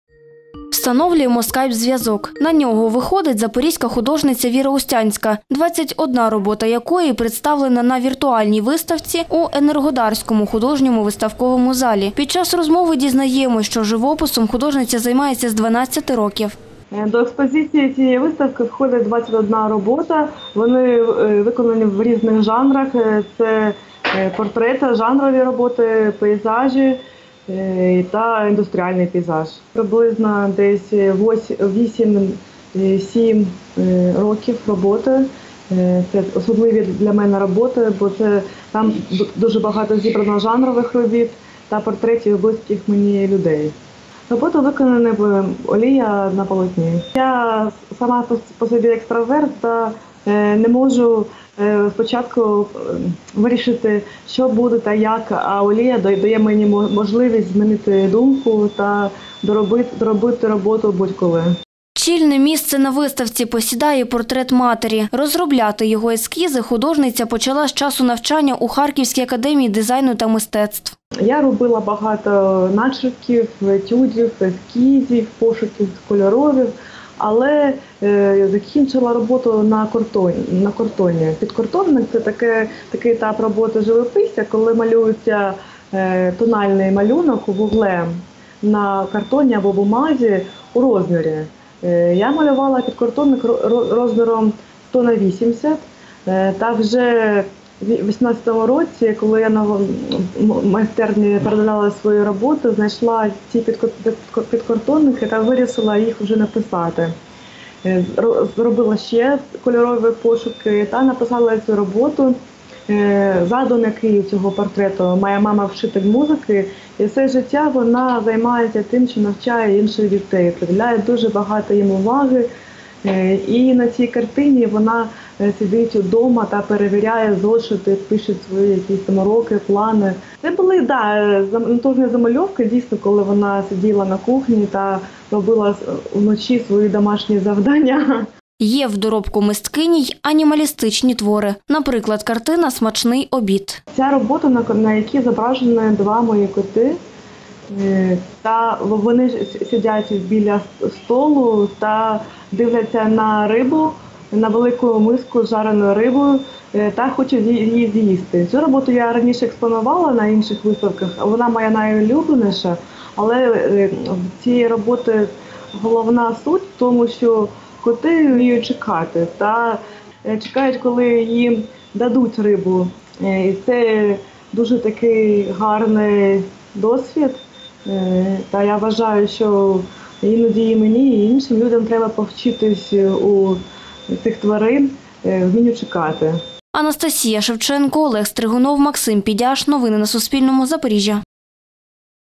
Новини - Віртуальна виставка Віри Устянської - 27.03.2020
Персональна виставка запорізької художниці Віри Устянської мала б відкритись в Енергодарському художньому виставковому залі у середині березня. Та через запровадження загальнонаціонального карантину, організатори представили її широкій публіці онлайн- у вигляді віртуальної екскурсії. З авторкою творів поспілкувалась Анастасія Шевченко.